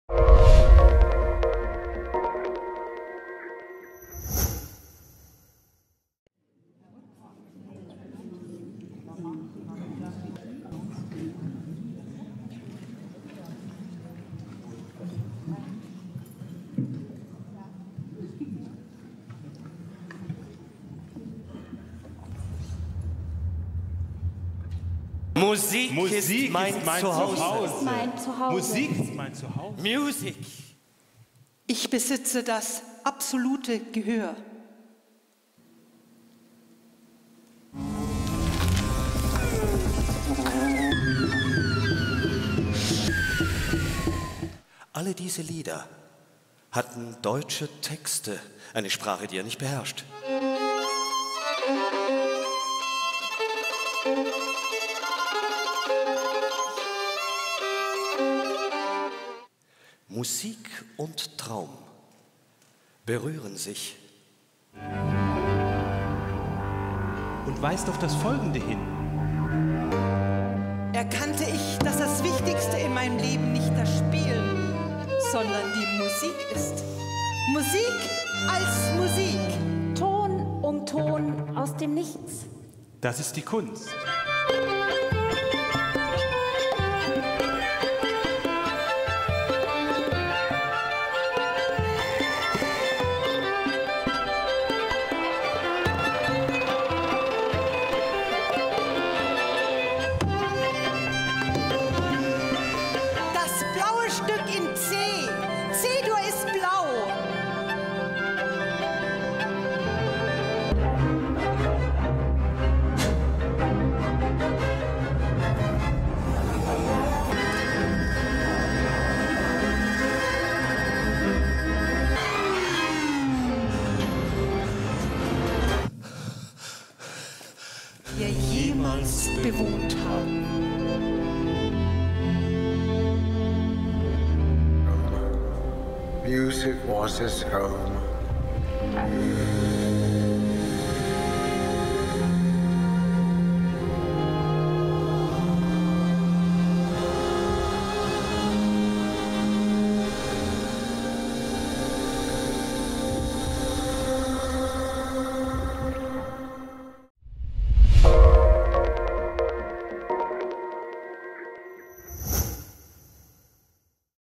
MUSICOPHILIA - Meta theater Munich - EXTRAITS
D'après l'oeuvre d'Oliver Sacks
Scène principale du théâtre Prospero
11 et 12 février 2015
Une production de META THEATER MUNICH
en collaboration avec LE GROUPE DE LA VEILLÉE